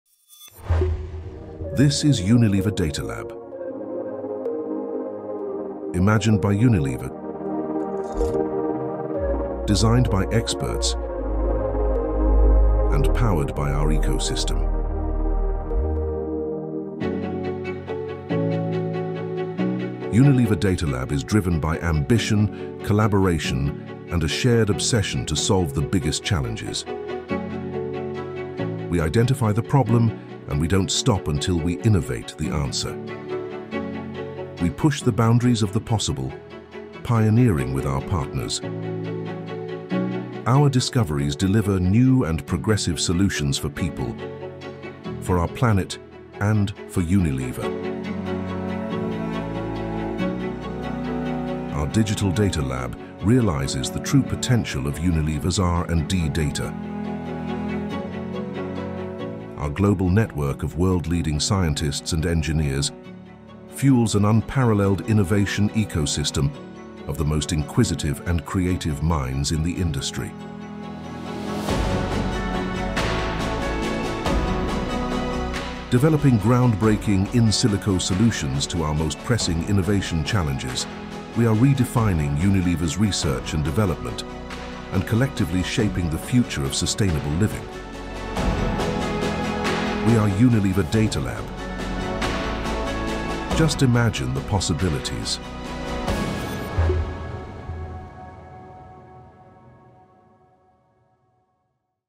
Unilever Research & Development’s DataLab Ecosystem Launch Video
This video provides a high-level view of Unilever Research and Development’s DataLab Ecosystem and its current Ecosystem Partners.

Discover how Unilever’s global network of world-leading digital scientists and engineers is fuelling an unparalleled innovation ecosystem of the most inquisitive and creative minds in the industry.